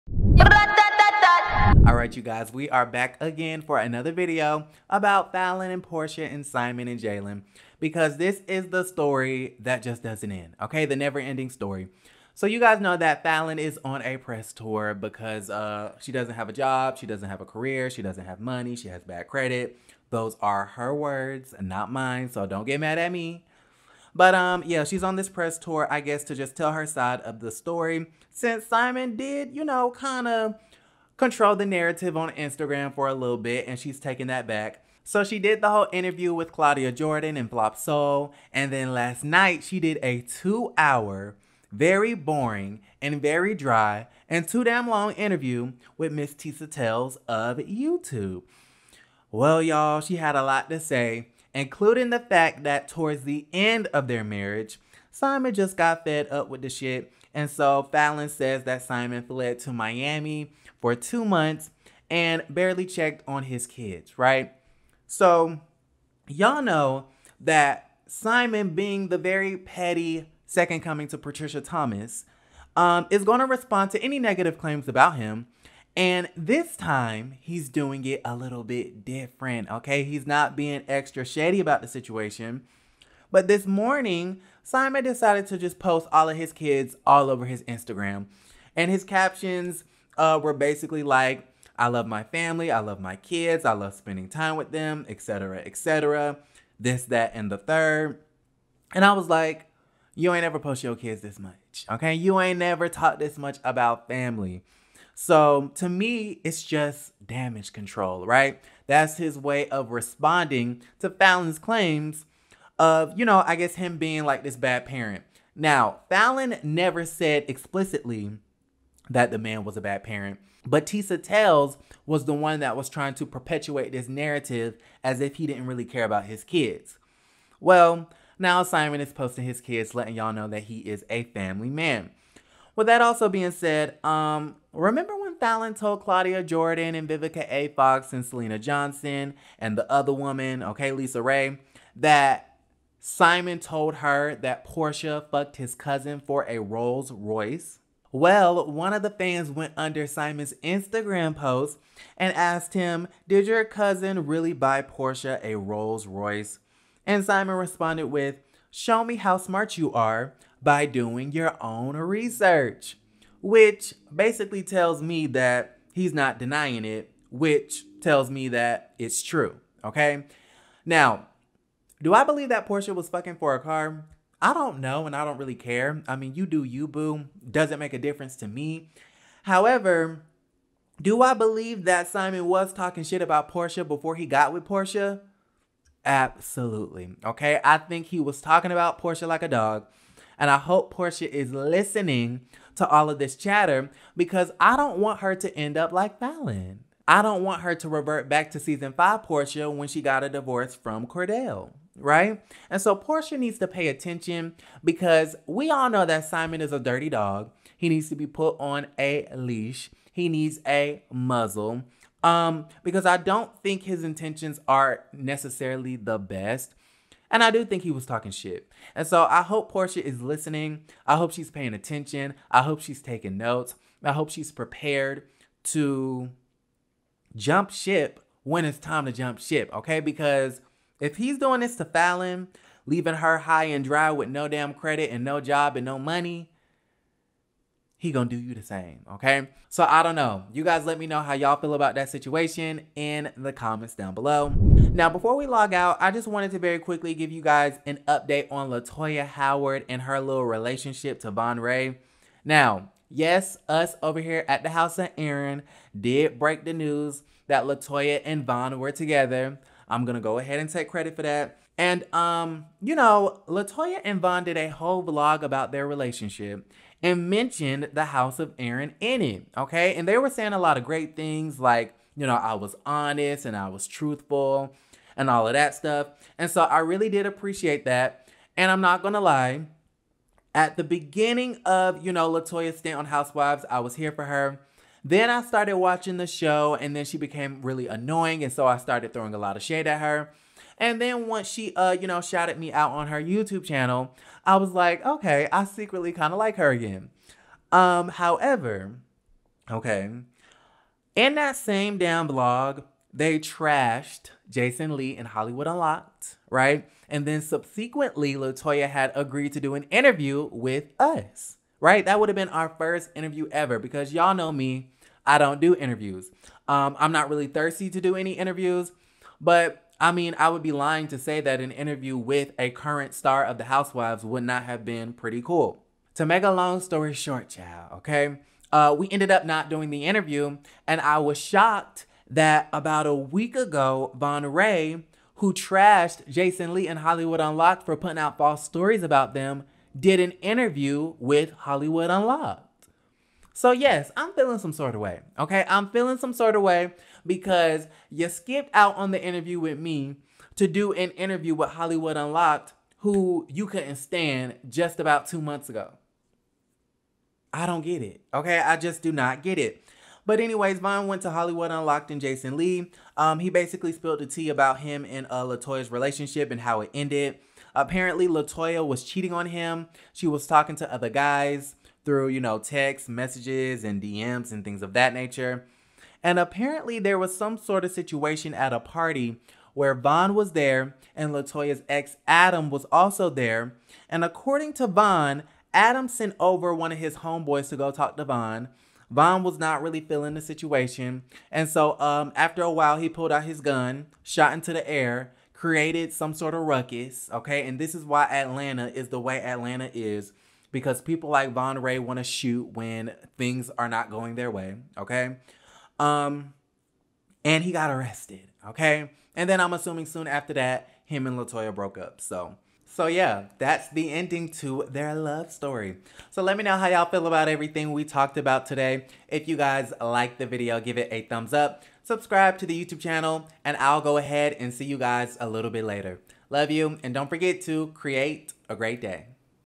Latoya and Von Diss Haus of Aaron for Jason Lee, Simon Guobadia Drama
Latoya and Von diss Haus of Aaron to do an interview with Jason lee. Falynn and Simon continue with more drama.

$aaronekeenan